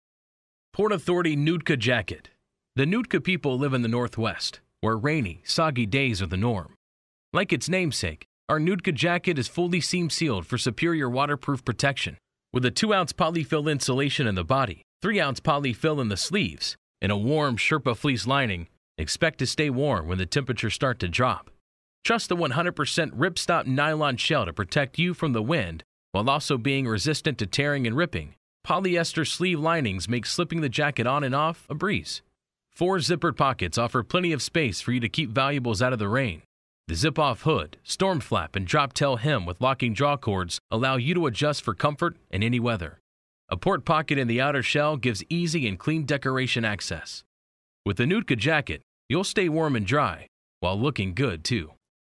Port Authority J792 Nootka Jacket - Buy at ApparelnBags.com
Port Authority J792 Nootka Jacket

Feature:

• 100% ripstop Nylon shell with Taslan Nylon accents 1500MM fabric waterproof rating
• Seam-sealed for added waterproof protection
• 2-ounce polyfill insulation in body 3-ounce polyfill in sleeves
• 100% Polyester Sherpa Fleece body lining
• Nylon sleeve lining for easy on/off Zip-off hood Storm flap
• Four zippered pockets Drop tail hem with Drawcord and toggle for adjustability
• Port Pocket for easy Embroidery access
• Available Sizes: XS, S, M, L, XL, 2XL, 3XL, 4XL.